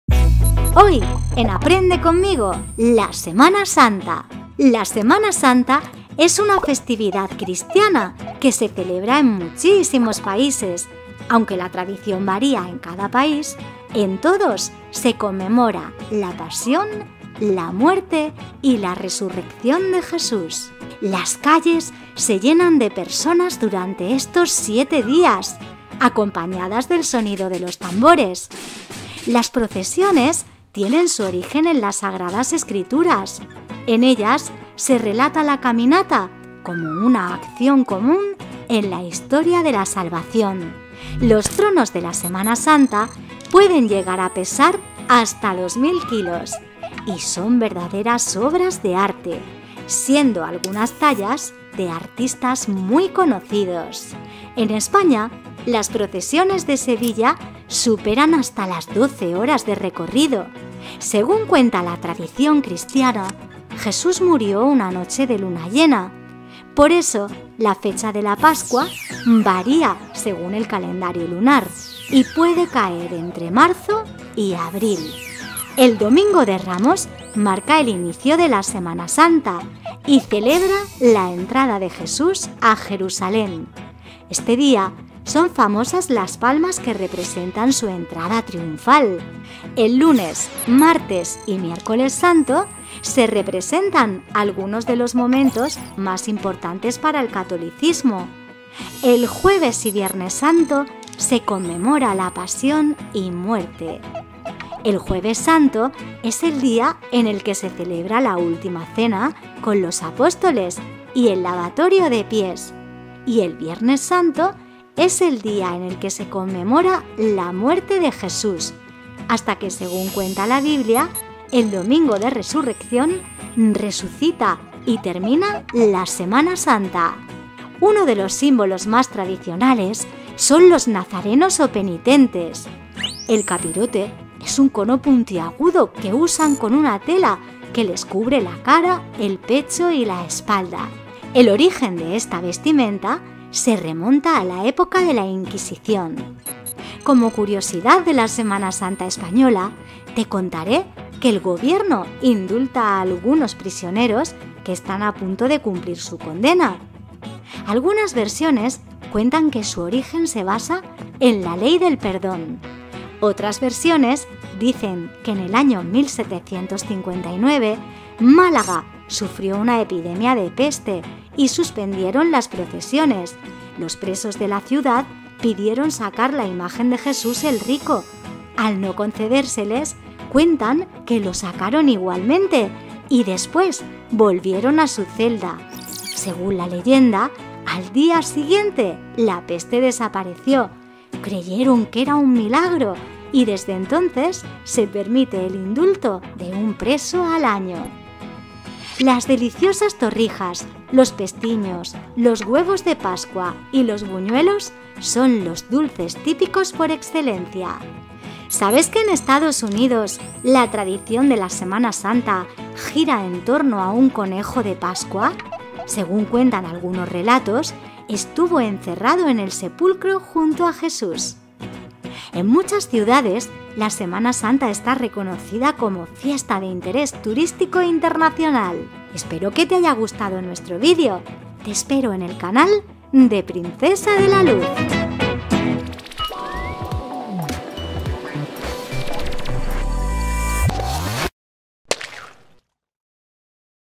¿Qué es la SEMANA SANTA❓ Explicación PARA Niños ✝️
La Semana Santa es una festividad cristiana que se celebra en muchísimos países aunque la tradición varía en cada país, en todos se conmemora la pasión, la muerte y la resurrección de Jesús.

Las calles se llenan de personas durante estos siete días,  acompañadas
del sonido de los tambores.
Las procesiones tienen su origen en la Sagradas Escrituras, en ellas se relata la caminata como una acción común en la historia de la salvación.

Los tronos de la Semana Santa, pueden llegar a pesar hasta 2000 kilos y son verdaderas obras de arte, siendo algunas tallas de artistas muy conocidos.

En España, las procesiones de Sevilla superan hasta las 12 horas de recorrido.

Según cuenta La tradición Cristiana, Jesús murió una noche de luna llena por eso la fecha de la Pascua varía según el calendario lunar y puede caer entre marzo y abril.
El Domingo de Ramos marca el inicio de la Semana Santa y celebra la entrada de Jesús a Jerusalén, este día son famosas las palmas que representan su entrada triunfal.

El lunes, martes y Miércoles Santo se representan algunos de los momentos más importantes para el catolicismo.
El jueves y Viernes Santo se conmemora la pasión y muerte.
El Jueves Santo es el día en el que se celebra la última cena con los apóstoles y el lavatorio de pies y el Viernes Santo, es el día en el que se conmemora la muerte de Jesús  hasta que, según cuenta La Biblia, el Domingo de Resurrección resucita y termina la Semana Santa.

Uno de los símbolos más tradicionales son los nazarenos o penitentes. El capirote es un cono puntiagudo que usan con una tela que les cubre la cara, el pecho y la espalda. El origen de esta vestimenta se remonta a la época de la inquisición.

Como curiosidad de la Semana Santa española te contaré que el gobierno indulta a algunos prisioneros que están a punto de cumplir su condena.
Algunas versiones cuentan que su origen se basa en la ley del perdón, otras versiones dicen que en el año 1759 Málaga sufrió una epidemia de peste y suspendieron las procesiones. Los presos de la ciudad pidieron sacar la imagen de Jesús el Rico, al no concedérsele cuentan que lo sacaron igualmente y después volvieron a su celda. Según la leyenda al día siguiente la peste desapareció. Creyeron que era un milagro y desde entonces se permite el indulto de un preso al año.

Las deliciosas torrijas, los pestiños, los huevos de Pascua y los buñuelos, son los dulces típicos por excelencia.

¿Sabes qué En Estados Unidos la tradición de la Semana Santa gira en torno a un conejo de Pascua?
Según cuentan algunos relatos, estuvo encerrado en el sepulcro junto a Jesús.

En muchas ciudades, la Semana Santa está reconocida como Fiesta de Interés Turístico Internacional.

Espero que te haya gustado nuestro vídeo
Te espero en el canal de Princesa de La Luz.